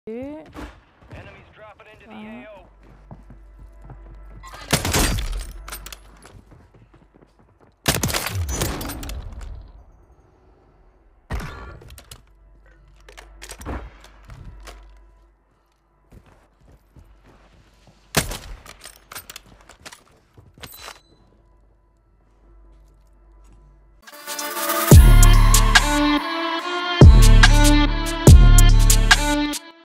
Kalei Couldn't Hide Excitement After This Insane Kill! 😲
2clicks does not own the content uploaded onto the channel.